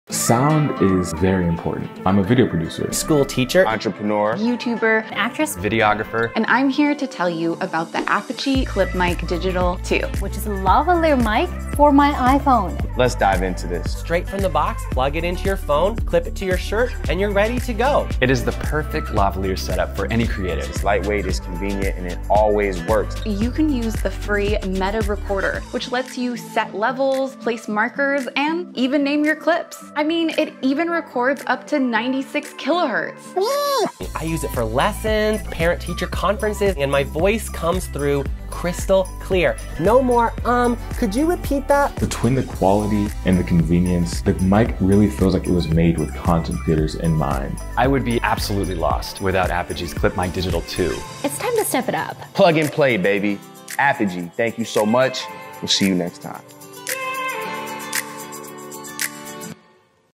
Videographers, Teachers, Actresses, YouTubers & more sound-off about Apogee ClipMic and MetaRecorder
People all over the world are using ClipMic to improve the audio of the content they’re creating.  Here are just a few of the people who are making the most of the Apogee ClipMic digital 2.

Transcript
Sound is very important. I'm a video producer.
School teacher.
Entrepreneur.
YouTuber.
Actress.
Videographer.
And I'm here to tell you about the Apogee ClipMic Digital 2, which is a lavalier mic for my iPhone.
Let's dive into this.
Straight from the box, plug it into your phone, clip it to your shirt, and you're ready to go.
It is the perfect lavalier setup for any creator. It's lightweight, it's convenient, and it always works.
You can use the free MetaRecorder, which lets you set levels, place markers, and even name your clips. I mean, it even records up to 96 kilohertz.
I use it for lessons, parent-teacher conferences, and my voice comes through crystal clear. No more, "Um, could you repeat that?"
Between the quality and the convenience, the mic really feels like it was made with content creators in mind.
I would be absolutely lost without Apogee's ClipMic Digital 2.
It's time to step it up.
Plug and play, baby. Apogee, thank you so much.